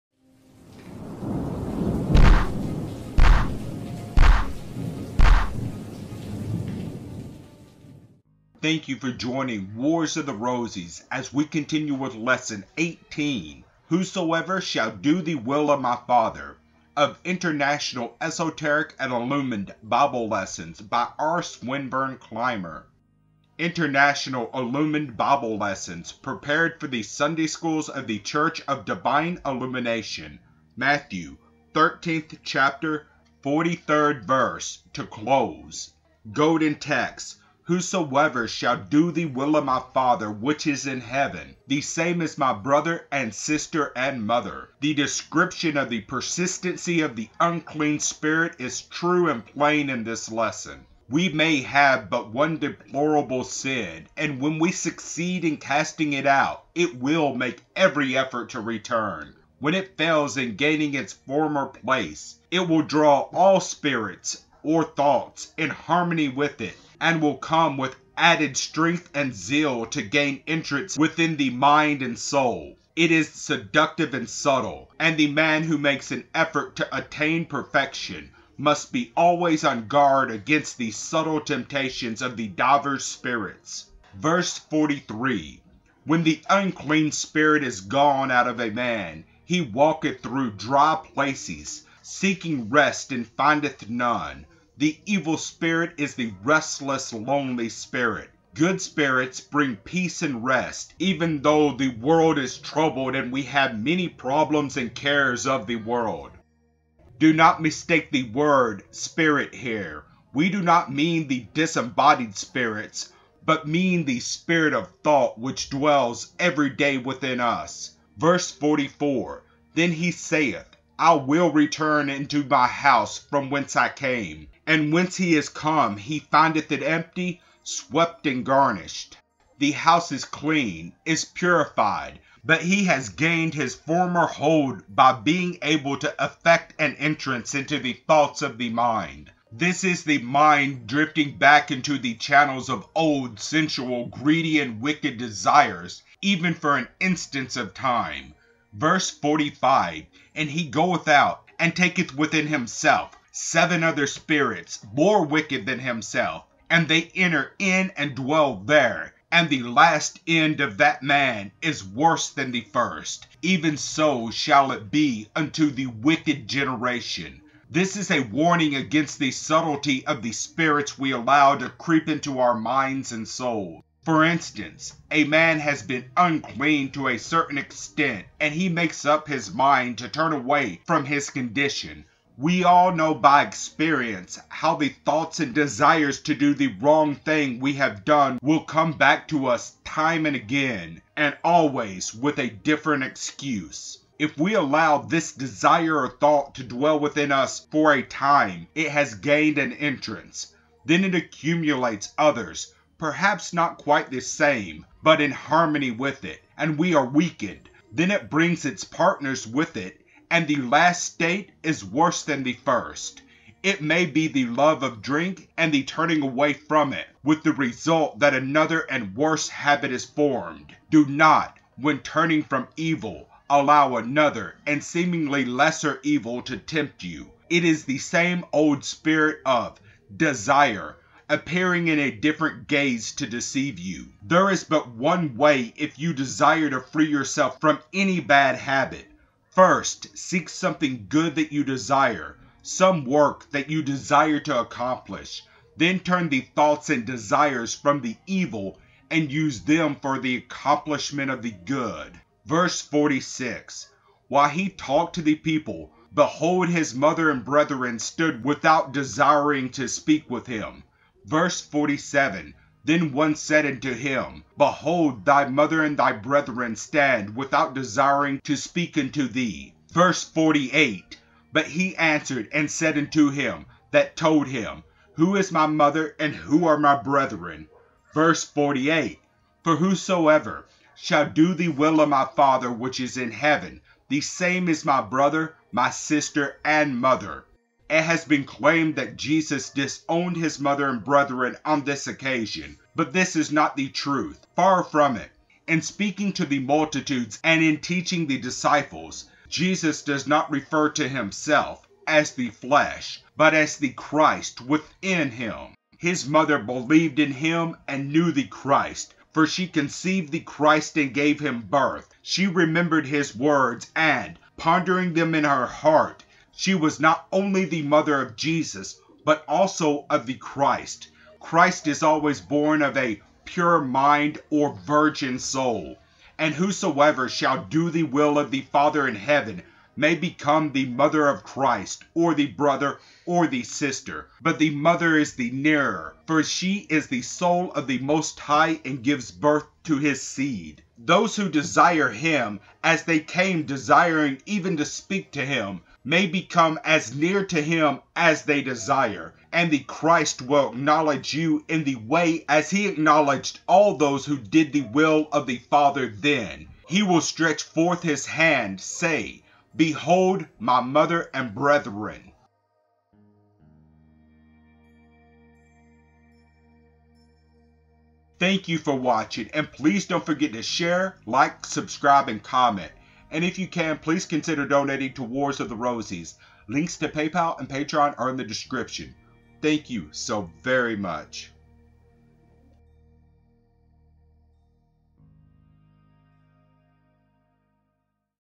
Whosoever Shall Do The Will Of My Father: International Esoteric & Illuminated Bible Lessons 18/63
“There is but one way if you desire to free yourself from any bad habit. First seek something good that you desire, some work that you desire to accomplish, then turn the thoughts and desires from the evil and use them for the accomplishment of the good.” Whosoever Shall Do The Will Of My Father, lesson 18 of 63 of International Esoteric & Illuminated Bible Lessons Prepared for the Sunday Schools of the Church of Divine Illumination series by R. Swinburne Clymer.